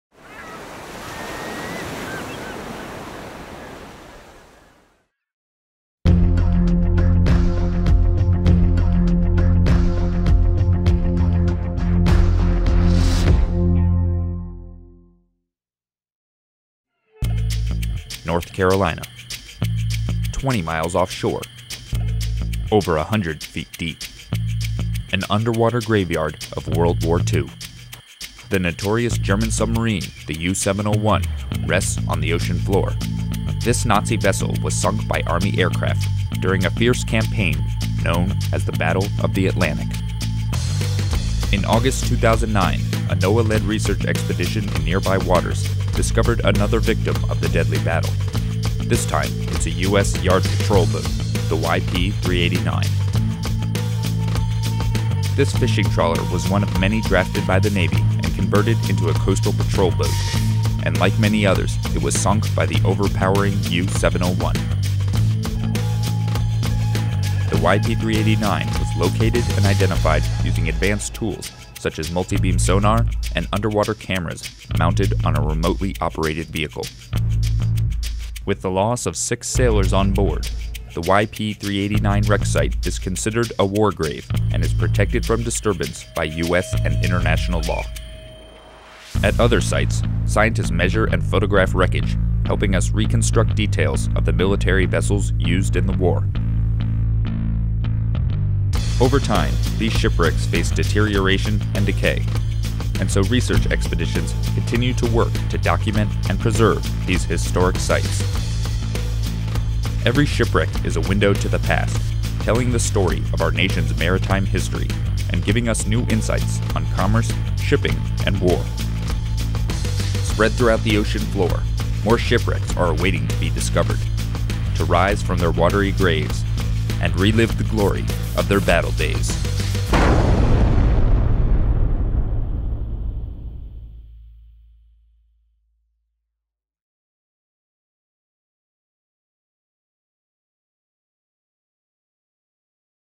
Battle of the Atlantic
North Carolina… twenty miles offshore… over a hundred feet deep – An underwater graveyard of WWII.

The notorious German submarine, the U701, rests on the ocean floor.  This Nazi vessel was sunk by Army aircraft during a fierce campaign known as the Battle of the Atlantic.

climate science and research. These videos are a free resource and are available on our website at oceantoday.noaa.gov.